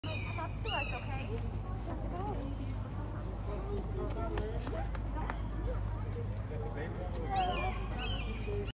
Baby Giraffe with Juvenile Giraffe
This baby was born out in the open in front of many people! Here it is about a month old. San Diego Zoo, California.